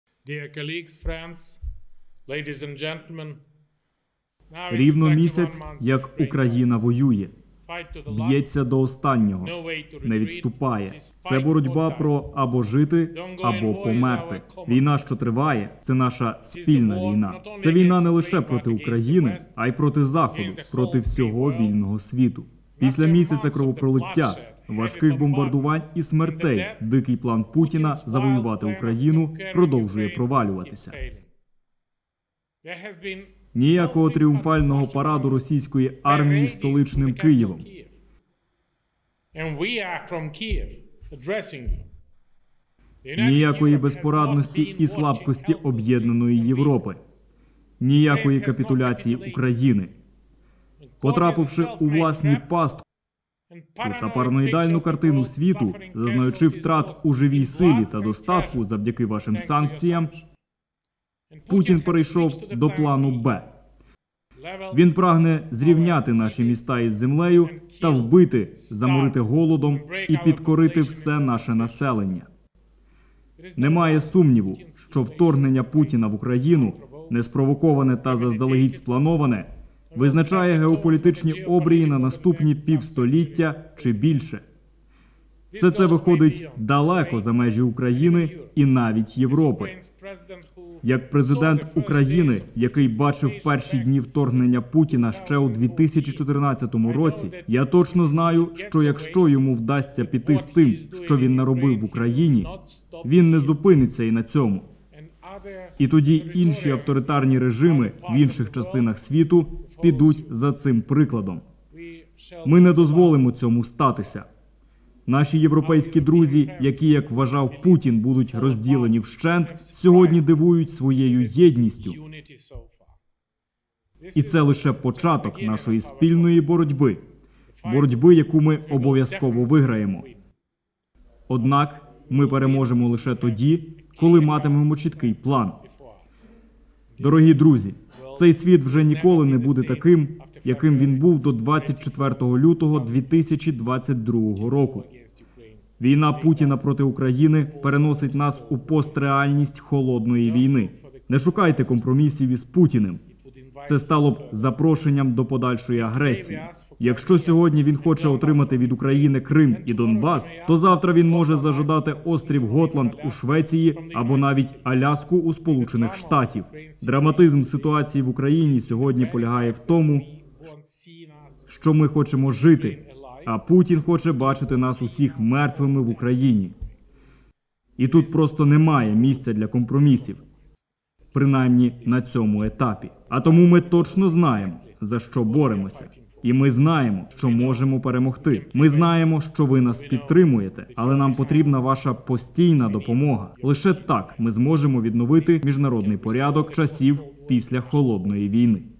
‼️ Ніякої капітуляції України. Це боротьба про або жити, або померти
Рівно місяць, як Україна воює. Бʼється до останнього. Не відступає. Це боротьба про або жити, або померти. Війна, що триває, це наша спільна війна. Це війна не лише проти України, а й проти Заходу, проти всього Вільного світу.

Після місяця кровопролиття, важких бомбардувань і смертей дикий план путіна завоювати Україну продовжує провалюватися. Ніякого тріумфального параду російської армії столичним Києвом. Ніякої безпорадності і слабкості Об’єднаної Європи. Ніякої капітуляції України.